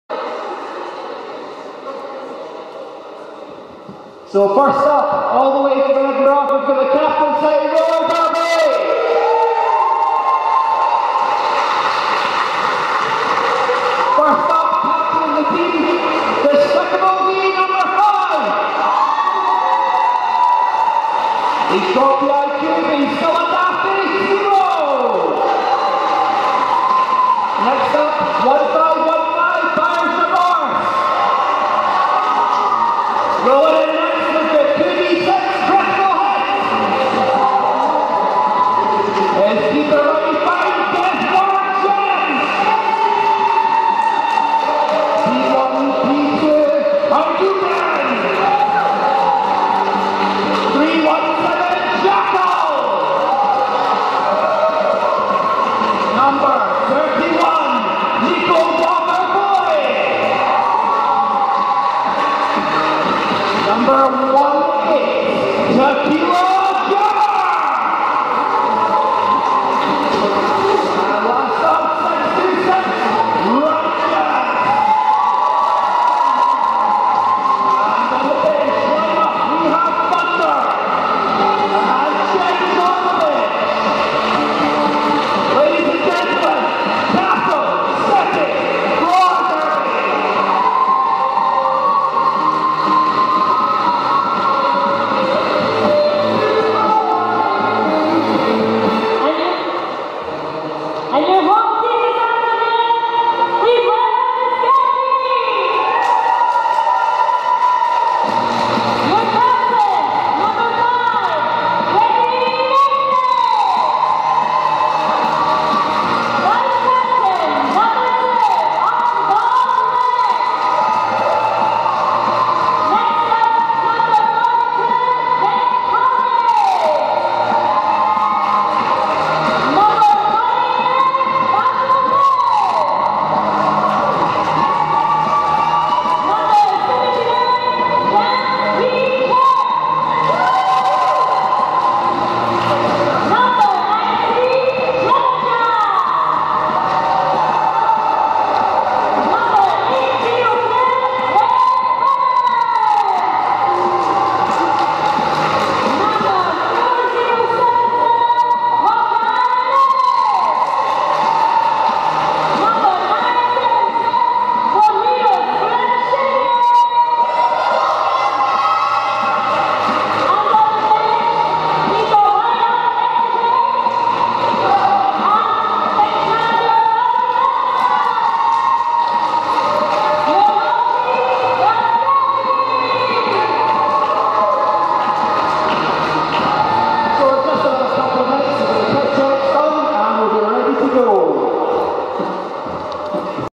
Scottish Men's Roller Derby Nationals 2015 - Bout 1 Skateout
Bairn City Rollers' Skelpies v Capital City Roller Derby
First bout in the inaugural year of the Scottish Men's Roller Derby Nationals
This is the Skateout